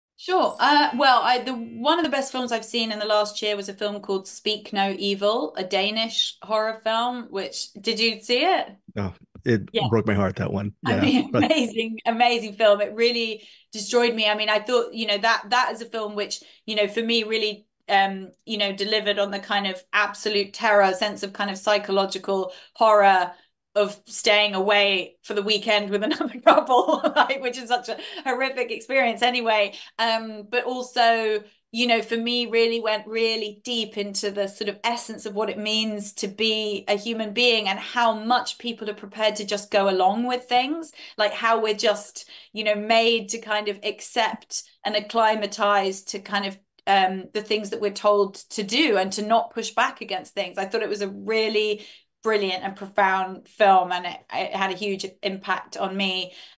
Romola Garai Was "Destroyed" By "Amazing" Movie 'SPEAK NO EVIL'
During the interviews for "One Life," I asked Romola Garai to name one of her favorite movies, and she picked "Speak No Evil." "One Life" was released in theaters stateside on March 15, 2024.

Let us know your thoughts on SpeakNoEvil and One Life.

Photos: IFC Midnight & Shudder